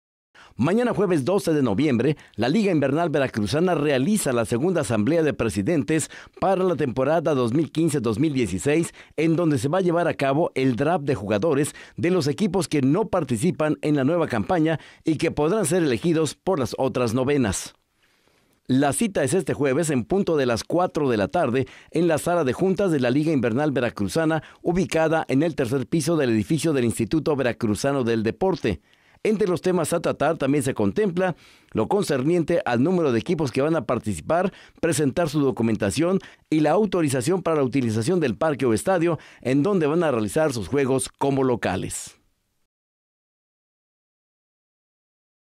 Junta LIV
EL VERACRUZANO HUMBERTO SOSA QUIEN PARTICIPA EN EL TORNEO PREMIER 12 CON MÉXICO NO SERÁ SANCIONADO POR LMB NI POR EL ÁGUILA POR SER CONVOCADO POR
LA FEDERACIÓN MEXICANA DE BÉISBOL.